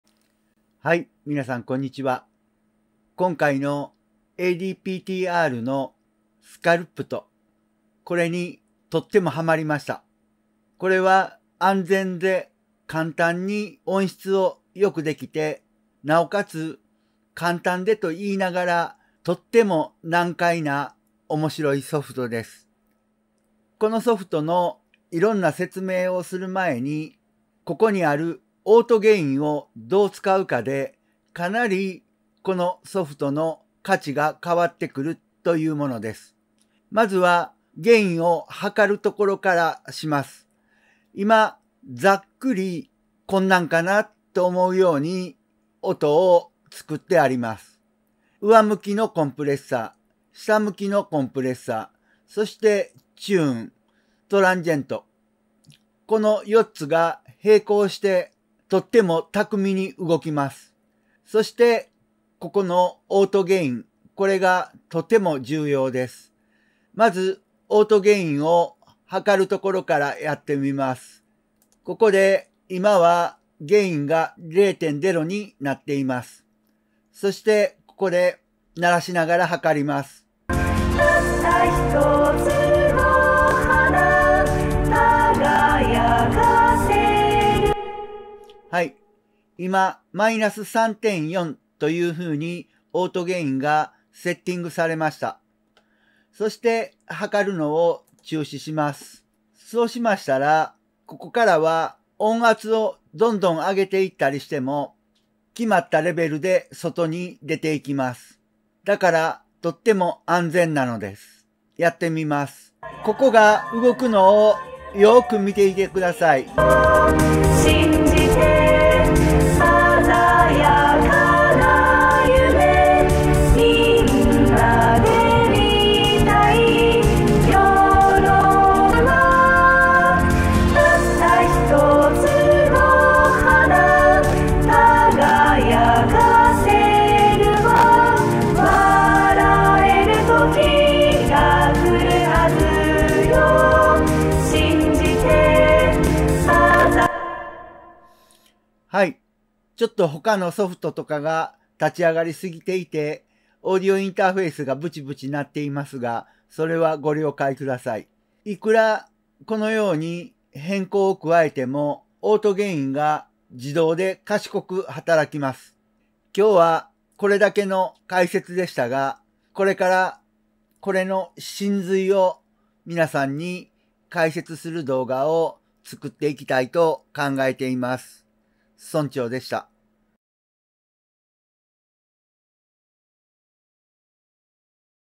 【Plugin Alliance新ソフト】ADPTR AUDIO SCULPTは怪物？魔物？ 感動の最強プラグイン ～序章編～オートゲインが素晴らしい
上向きのコンプってなんだ！！
まずはこのソフトはオートゲインを分析することから始めます
とっても簡単にトリートメント
なのに「とっても奥深い」最強プラグイン